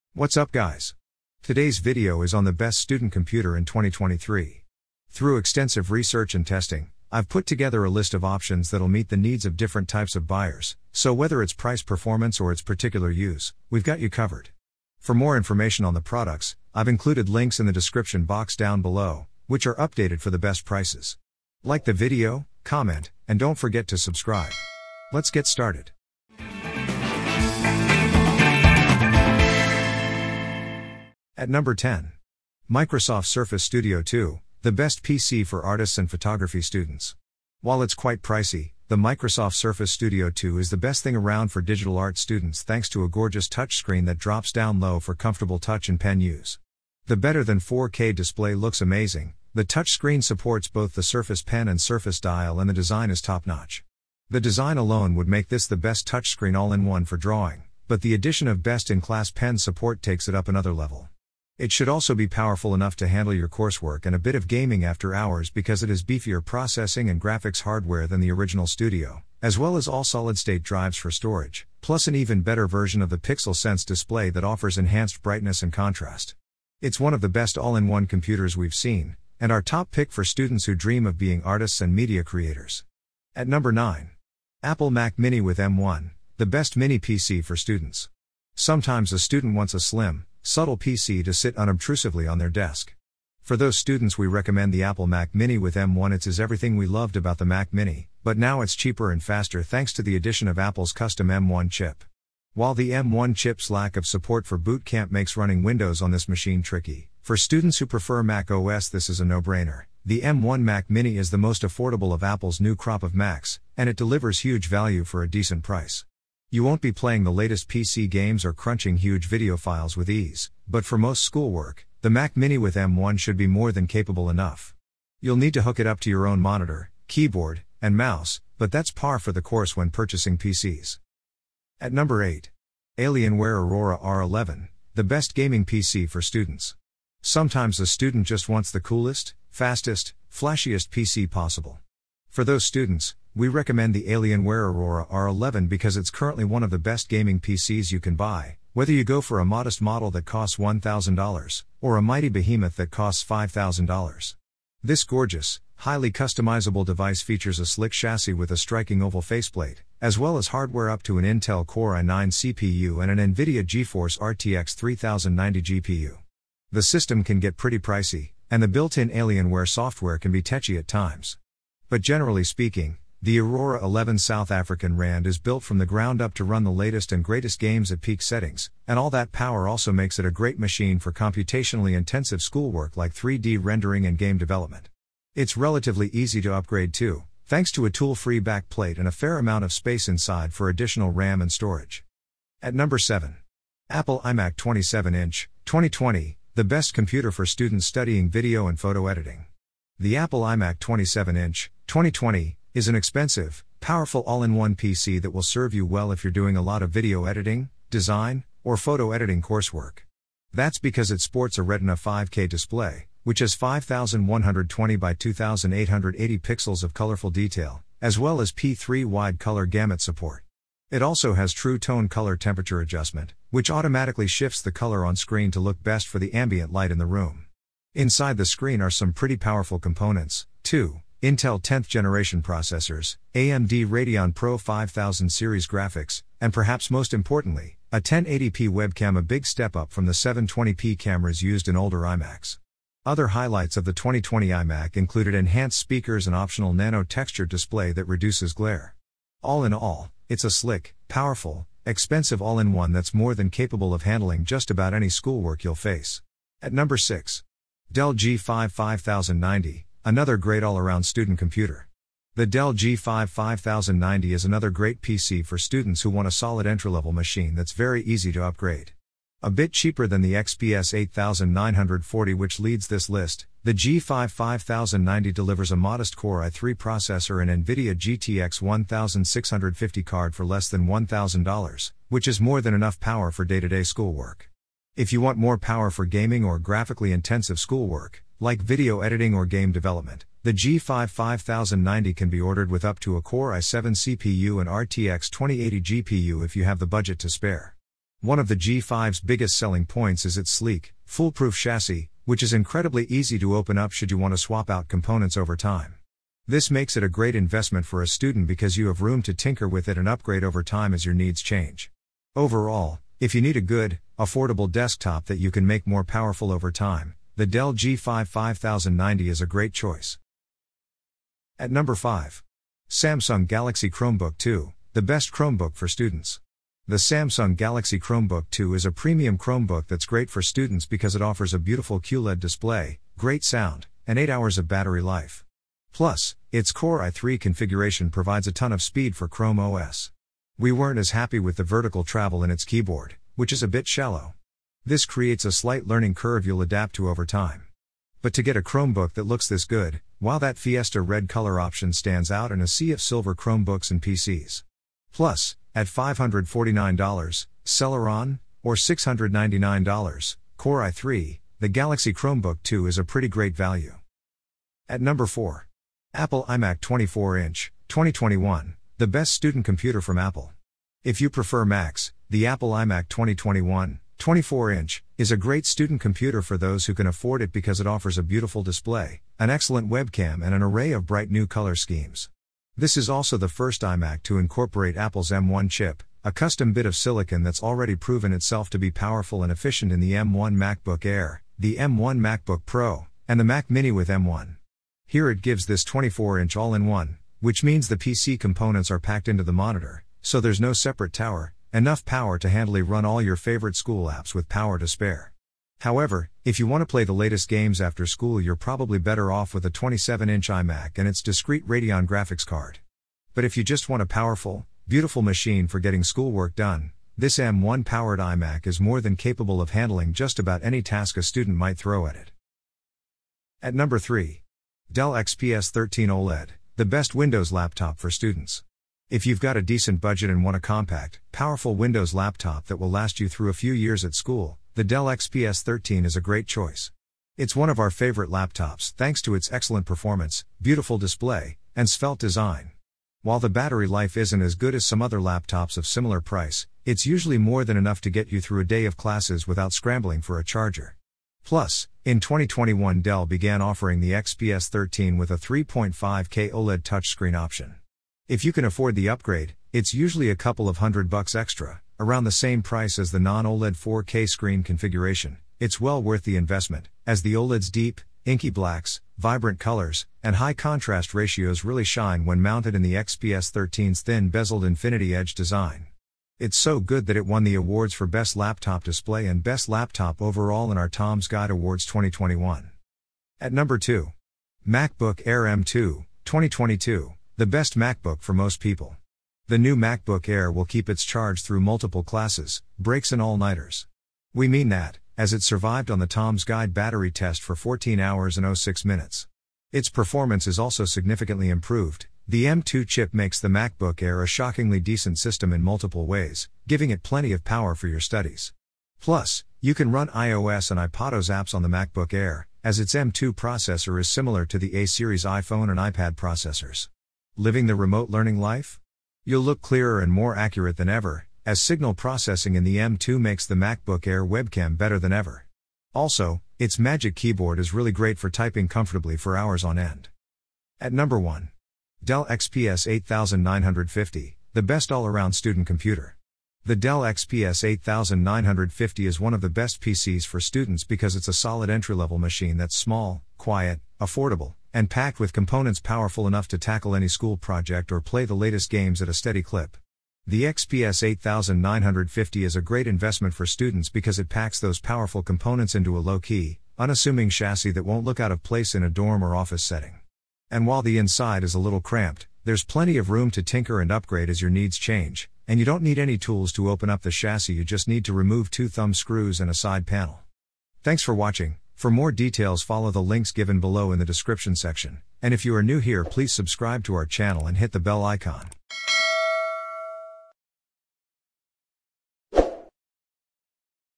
Top 10 Best student computer in 2023
➜10 Best Student Computer we listed in this video:
1. Dell XPS 8950: The best all-around student computer.

2. MacBook Air M2 (2022): The best MacBook for most people.

3. Dell XPS 13 OLED: The best Windows laptop for students.

4. Apple iMac 24-inch (2021): The best student computer from Apple.

5. Samsung Galaxy Chromebook 2: The best Chromebook for students.

6. Dell G5 5090: Another great all-around student computer.

7. Apple iMac 27-inch (2020): The best computer for students studying video and photo editing.

8. Alienware Aurora R11: The best gaming PC for students.

9. Apple Mac mini with M1: The best mini PC for students.

10. Microsoft Surface Studio 2: The best PC for artists and photography students.
Looking for the perfect computer to help you ace your academic work? Look no further! In this video, we'll be reviewing the 10 best student computers that are sure to cater to all your needs.

Starting at number 10, we have the Microsoft Surface Studio 2, which is the best PC for artists and photography students. Next up is the Apple Mac mini with M1, which is the best mini PC for students, coming in at number 9. At number 8, we have the Alienware Aurora R11, the best gaming PC for students.

For students studying video and photo editing, the Apple iMac 27-inch (2020) is the way to go and takes the seventh spot. At number six, we have the Dell G5 5090, which is another great all-around student computer.

If you're looking for the best Chromebook for students, then the Samsung Galaxy Chromebook 2 comes in at number five. The Apple iMac 24-inch (2021) is the fourth best student computer from Apple.

For the best Windows laptop for students, look no further than the Dell XPS 13 OLED, which takes the third spot. The MacBook Air M2 (2022) is the second best MacBook for most people. And finally, at number one, we have the Dell XPS 8950, which is the best all-around student computer.

So, there you have it, the top 10 best student computers on the market. We hope this video has helped you in making an informed decision about which computer will best suit your needs. Happy studying!